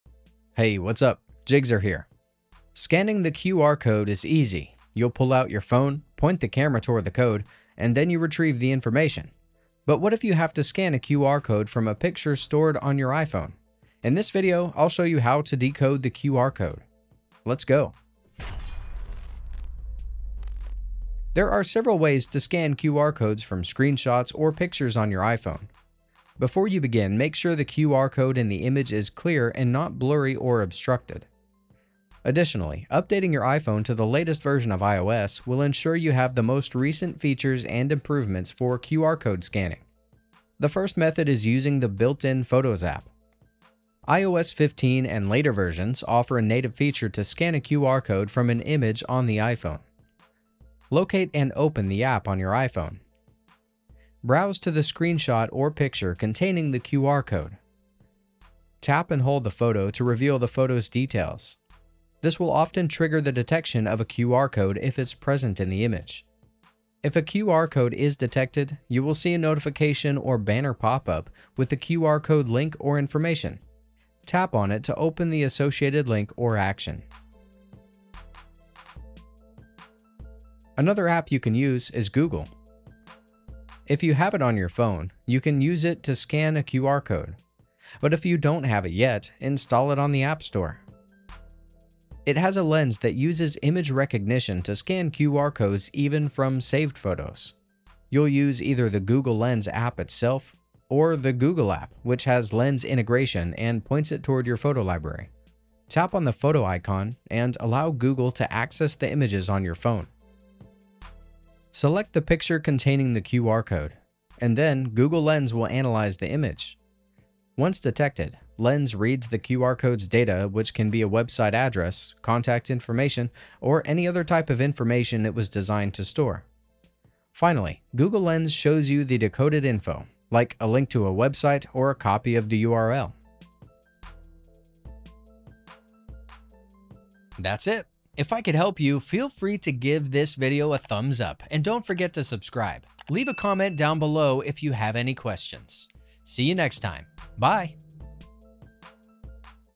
How To Scan QR Codes On Screenshot Or Picture On iPhone! [2024]
Scanning the QR code is easy. Simply take out your phone, point the camera at the code, and the information is retrieved almost instantly. However, scanning a QR code from a picture saved on your iPhone requires a different approach. In this video, I will guide you through the steps to successfully decode a QR code from an image in your photo library. Yours Jigxor

Timestamps:
00:00 | Scan using Photos app
01:20 | Google Lens

Video: 997